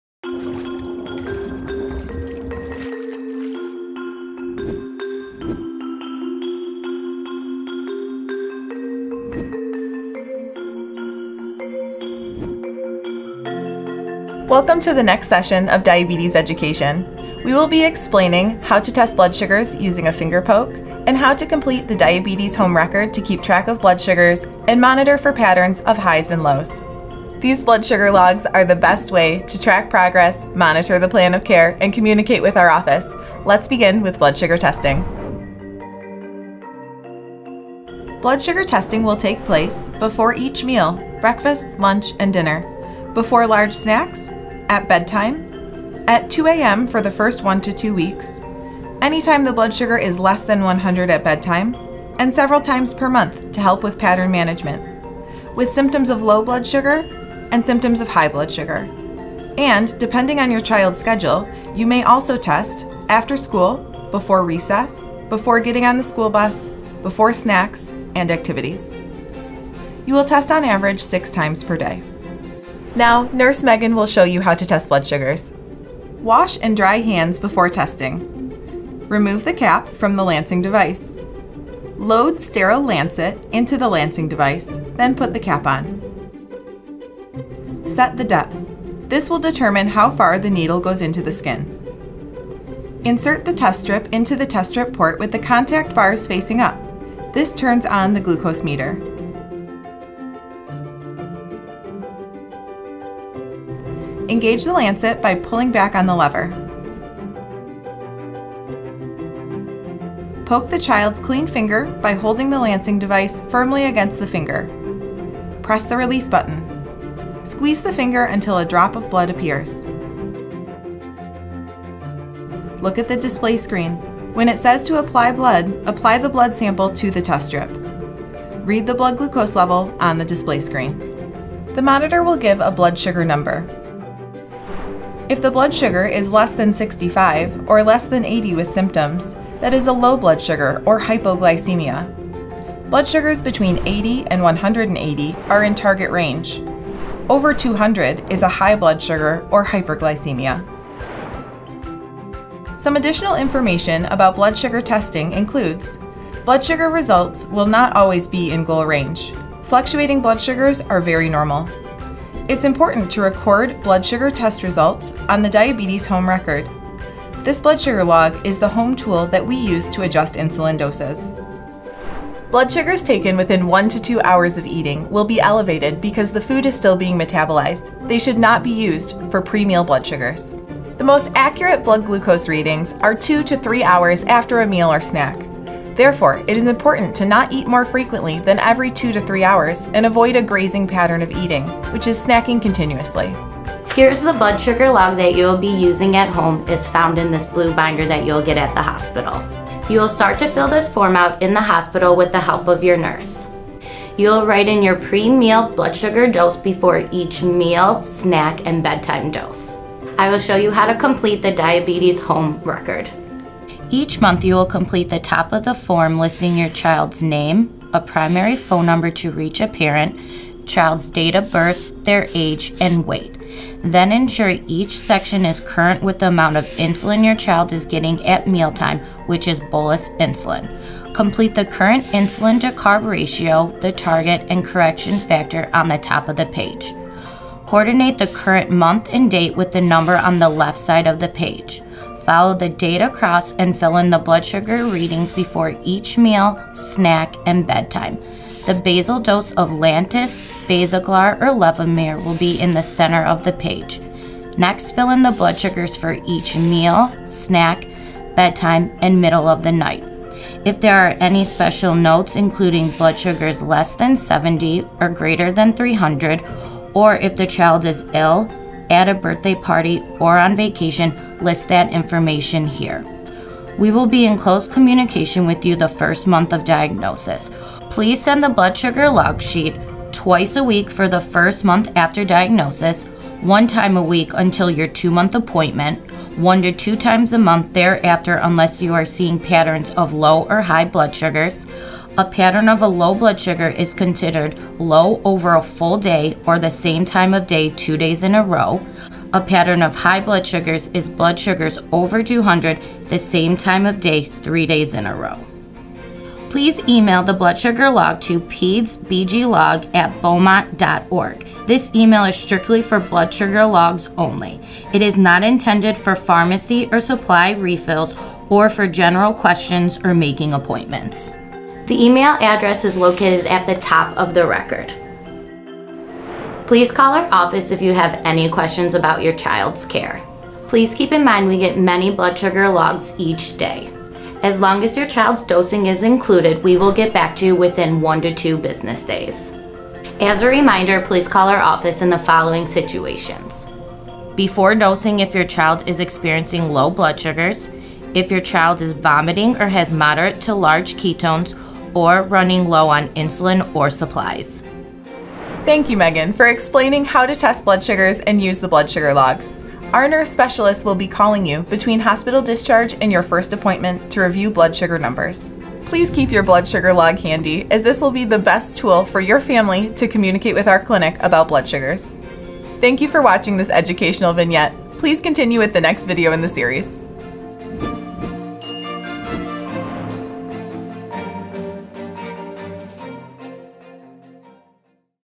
Blood Glucose Testing and Log Sheets | Managing Childhood Diabetes
This eight-part, new-onset diabetes video series is used by Beaumont Health, along with the Managing Childhood Diabetes binder and the hospital educational checklist. This video reviews how to use a lancing device and blood glucose meter to test blood sugars. It provides an explanation of how to complete the blood sugar log sheet, as well as how to utilize the log sheet to watch for patterns of high and low blood sugar trends.

NOTE: These videos are Beaumont guidelines to diabetes management. Please follow your child’s individualized care plan and call your endocrinologist with any questions.